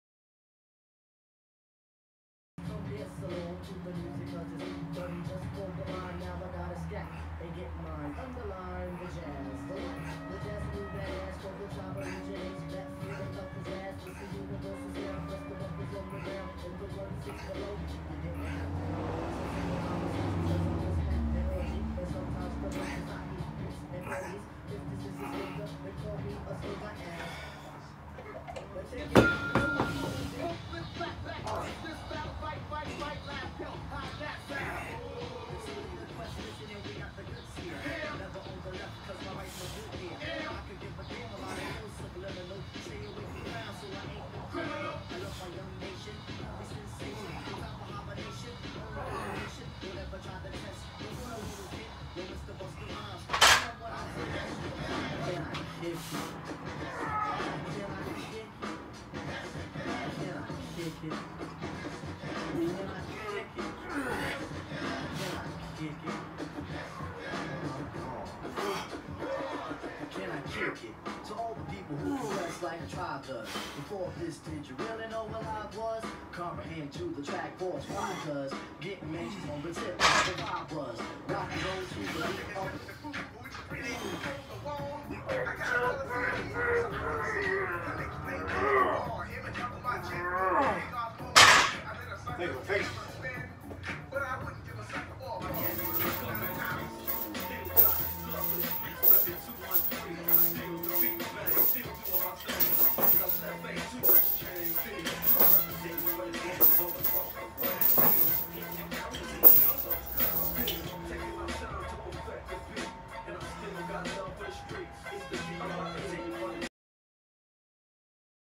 Hittin’ some biceps & triceps today! Team I Can! Can You?
Bicep curls & tricep extensions!